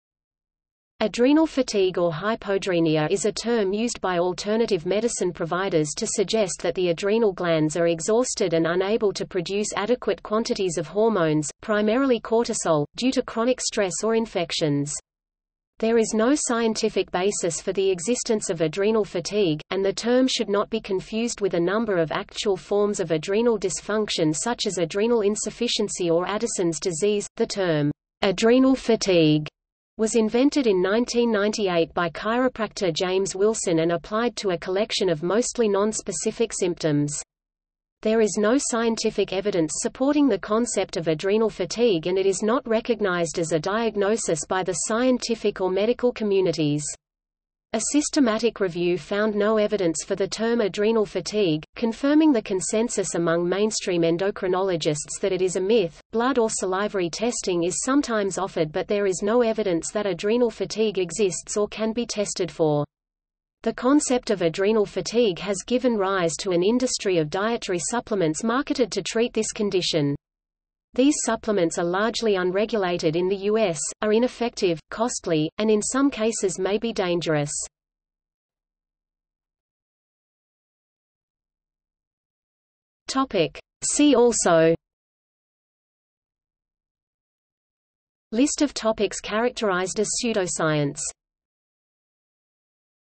Adrenal fatigue | Wikipedia audio article
00:01:19 See also

- Socrates

SUMMARY
Adrenal fatigue or hypoadrenia is a term used by alternative medicine providers to suggest that the adrenal glands are exhausted and unable to produce adequate quantities of hormones, primarily cortisol, due to chronic stress or infections. There is no scientific basis for the existence of adrenal fatigue, and the term should not be confused with a number of actual forms of adrenal dysfunction such as adrenal insufficiency or Addison's disease.The term "adrenal fatigue" was invented in 1998 by chiropractor James Wilson and applied to a collection of mostly non-specific symptoms.  There is no scientific evidence supporting the concept of adrenal fatigue and it is not recognized as a diagnosis by the scientific or medical communities.  A systematic review found no evidence for the term adrenal fatigue, confirming the consensus among mainstream endocrinologists that it is a myth.Blood or salivary testing is sometimes offered but there is no evidence that adrenal fatigue exists or can be tested for. The concept of adrenal fatigue has given rise to an industry of dietary supplements marketed to treat this condition. These supplements are largely unregulated in the U.S., are ineffective, costly, and in some cases may be dangerous.